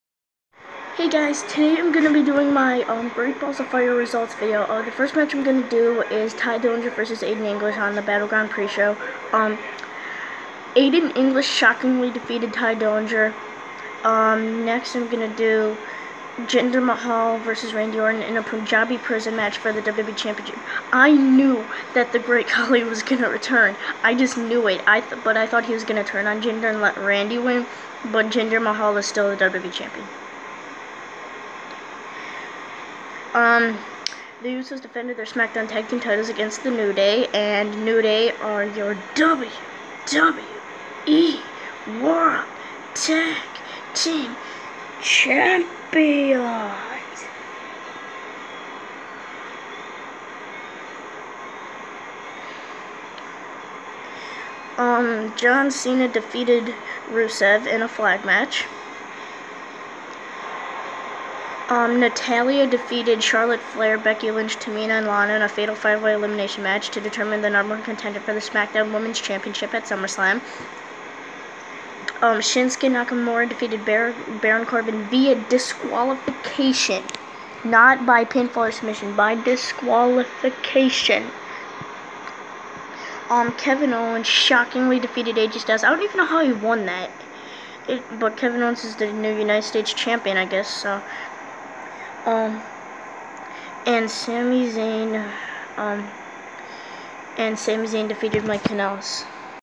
BattleGround 2017 PPV Results
Aiden English def Tye Dillinger
Jinder Mahal def Randy Orton for the WWE Championship
John Cena def Rusev
Natalya def Charlotte Flair, Becky Lynch, Tamina, and Lana to become the #1 Contender for the SmackDown Women's Championship
Shinsuke Nakamura def Baron Corbin via DQ
Kevin Owens def AJ Styles for the US Championship
Sami Zayn def Mike Kanellis